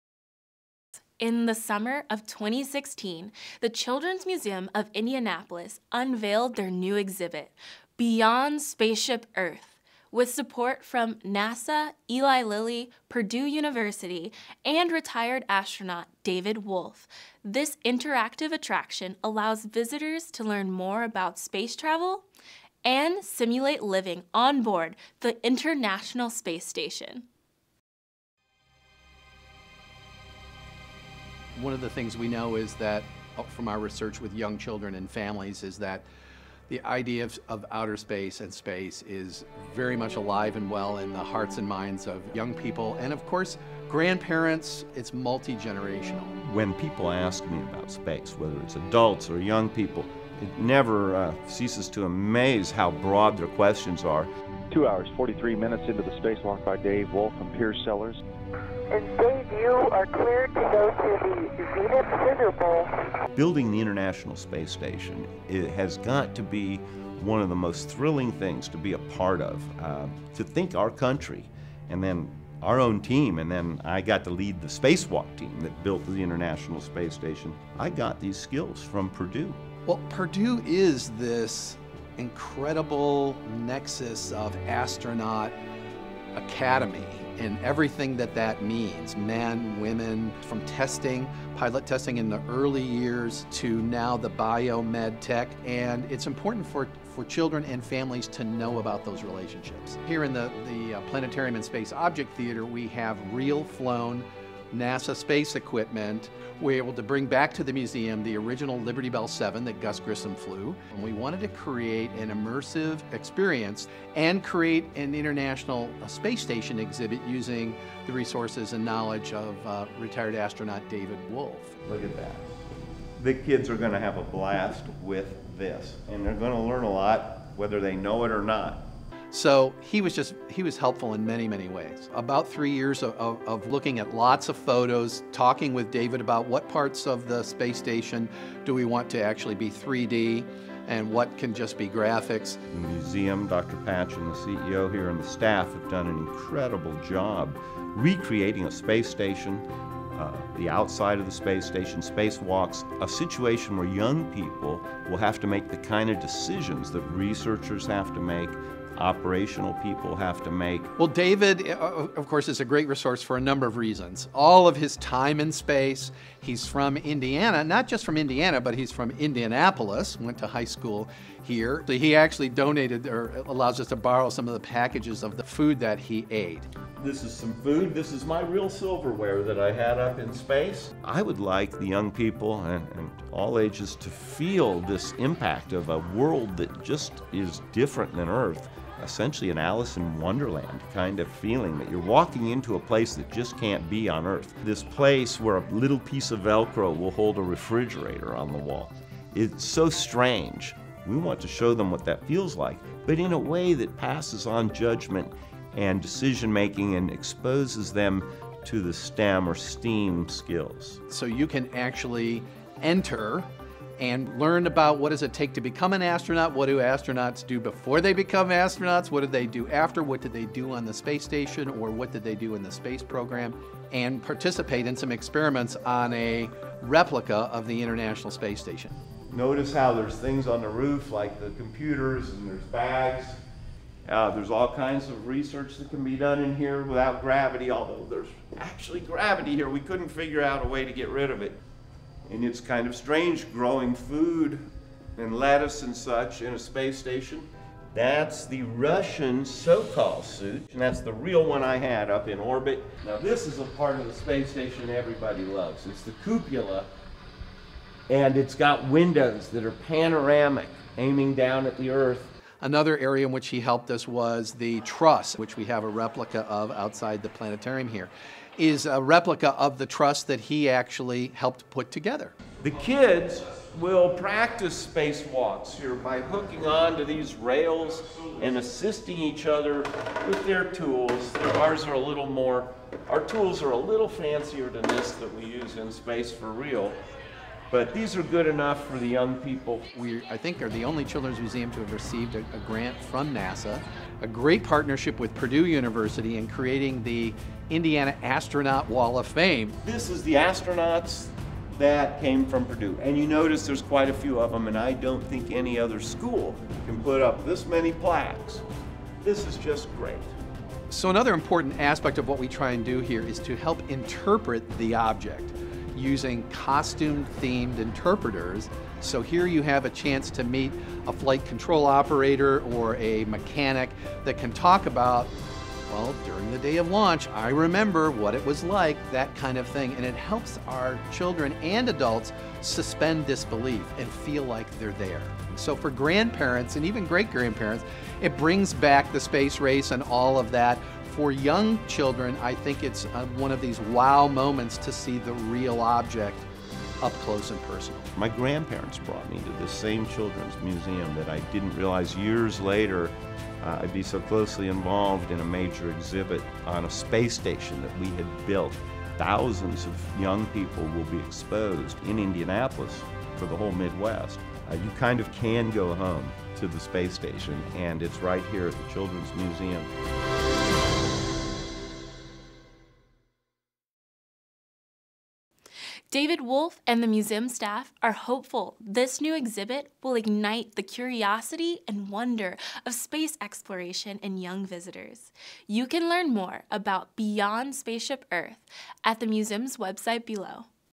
Boiler Bytes: "Beyond Spaceship Earth" exhibit
With support from NASA, Eli Lilly, Purdue University, and retired-astronaut David Wolf, the “Beyond Spaceship Earth” interactive attraction at the Children’s Museum of Indianapolis allows visitors to learn more about space travel and simulate living onboard the International Space Station!